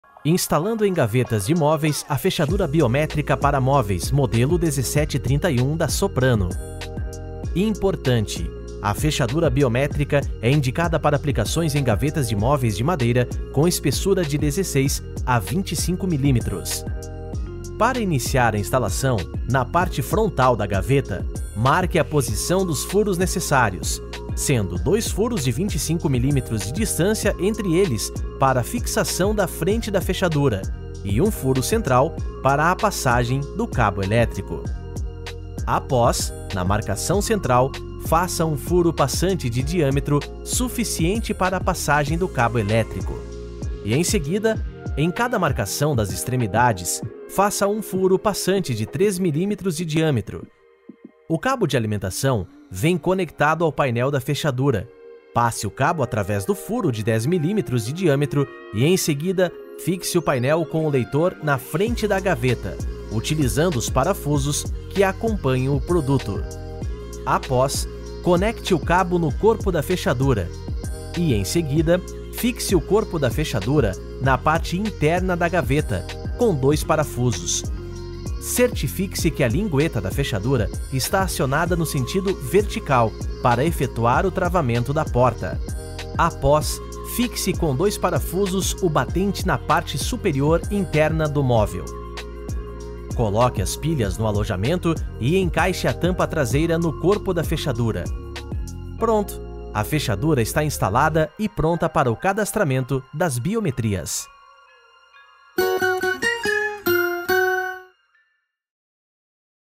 Como INSTALAR a Fechadura Biométrica para Móveis 1731 da Soprano em GAVETAS? OFICIAL
Perfeita para quem busca exclusividade e controle total sobre o acesso a gavetas e armários em lares e escritórios, conheça a praticidade e segurança da Fechadura Biométrica para Móveis 1731 da Soprano.

Neste vídeo orientamos como instalar a Fechadura Biométrica para Móveis 1731 da Soprano em GAVETAS.